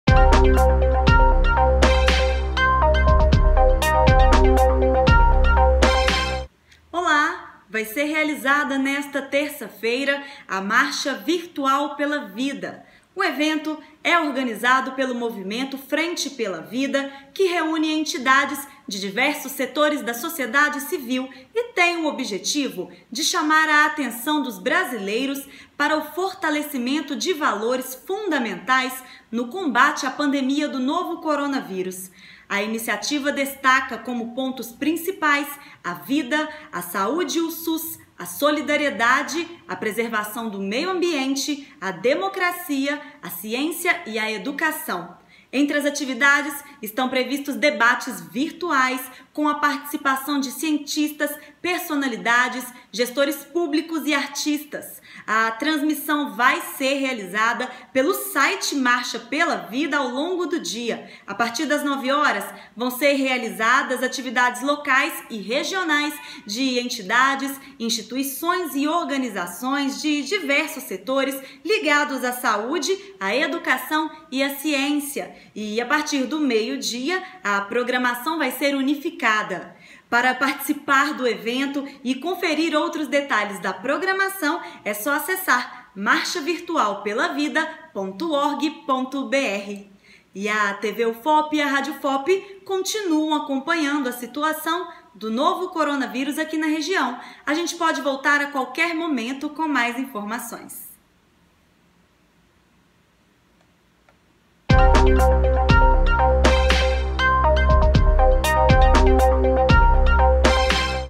Boletim TV UFOP - Marcha Virtual pela Vida chama a atenção para a importância da saúde e da ciência
Ação é organizada pelo movimento Frente pela Vida e será realizada nesta terça-feira (9) totalmente on-line. A iniciativa vai debater, também, temas como o SUS, a preservação do meio ambiente e a democracia. Confira os detalhes da programação e como participar do evento no boletim!

SIGA TV UFOP!
Facebook.com/TVUFOP
Instagram.com/TVUFOP